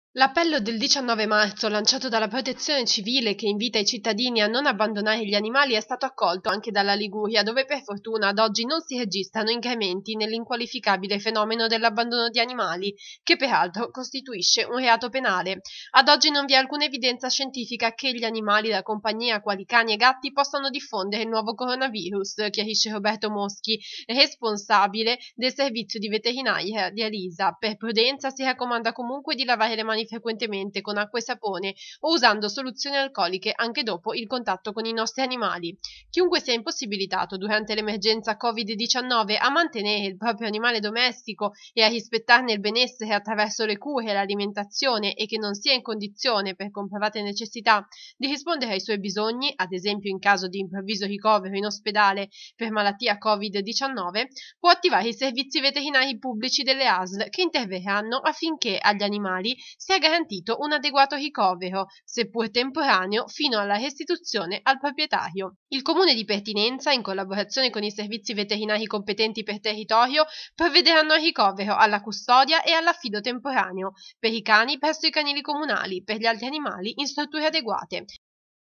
CORONAVIRUS: LA TUTELA DEL BENESSERE DEGLI ANIMALI DA COMPAGNIA IN UN MOMENTO DI EMERGENZA
L’appello del 19 marzo 2020 lanciato dalla Protezione civile, che invita i cittadini a non abbandonare gli animali, è stato accolto anche nella nostra regione, dove per fortuna, ad oggi, non si registrano incrementi dell’inqualificabile fenomeno dell’abbandono di animali, che peraltro costituisce un reato penale: «Ad oggi non vi è alcuna evidenza scientifica che gli animali da compagnia, quali cani e gatti, possano diffondere il nuovo coronavirus – chiarisce Roberto Moschi, responsabile del servizio di veterinaria di Alisa -. Per prudenza, si raccomanda comunque di lavare le mani frequentemente con acqua e sapone o usando soluzioni alcoliche anche dopo il contatto con i nostri animali. Chiunque sia impossibilitato durante l’emergenza Covid-19 a mantenere il proprio animale domestico e a rispettarne il benessere attraverso le cure, l’alimentazione e che non sia in condizione, per comprovate necessità, di rispondere ai suoi bisogni (ad esempio, in caso di improvviso ricovero in ospedale per malattia Covid-19), può attivare i servizi veterinari pubblici delle Asl che interverranno affinché agli animali sia garantito un adeguato ricovero, seppur temporaneo, fino alla restituzione al proprietario. Il Comune di pertinenza, in collaborazione con i servizi veterinari competenti per territorio, provvederanno al ricovero, alla custodia e all’affido temporaneo: per i cani, presso i canili comunali e, per gli altri animali, in strutture adeguate». Anche i servizi di smaltimento di carcasse di animali domestici sono attività indifferibili: «Qualora in questi giorni il vostro amico a quattro zampe dovesse lasciarvi, è possibile contattare i servizi veterinari della Asl di riferimento, che provvederà al ritiro delle spoglie del vostro animale per l'invio alla termodistruzione, in condizioni di sicurezza» conclude Moschi.

NUMERI UTILI SERVIZI VETERINARI ASL - AREA DI SANITÀ ANIMALE